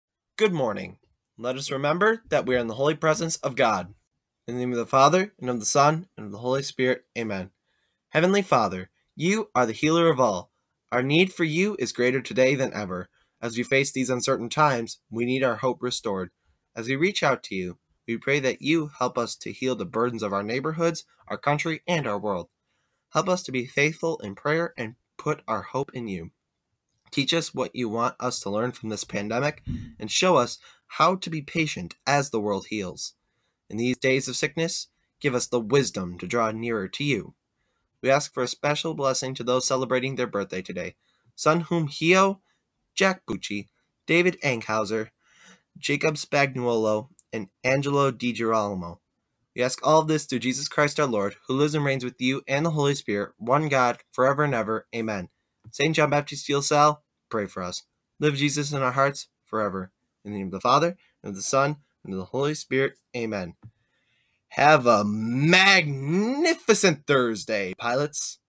Morning Prayer 5.7.20
Morning prayer for Thursday, May 7, 2020, read by Thomas Marsh, '21.

Heavenly Father,
You are the healer of all.  Our need for You is greater today than ever.  As we face these uncertain times, we need our hope restored.  As we reach out to You, we pray that You help us to heal the burdens of our neighborhoods, our country and our world.  Help us to be faithful in prayer and put our hope in You.  Teach us what you want us to learn from this pandemic and show us how to be patient as the world heals.  In these days of sickness, give us the wisdom to draw nearer to You. We ask for a special blessing to those celebrating their birthday today: Sunhuem Heo, Jack Bucci, David Enghauer, Jacob Spagnuolo, and Angelo DiGirolamo. We ask all of this through Jesus Christ our Lord who lives and reigns with you and the Holy Spirit one God, forever and ever.  Amen.

Saint John Baptist De La Salle . . . . (Pray for us)
Live Jesus in our hearts……(Forever!)

Have a great day, Pilots!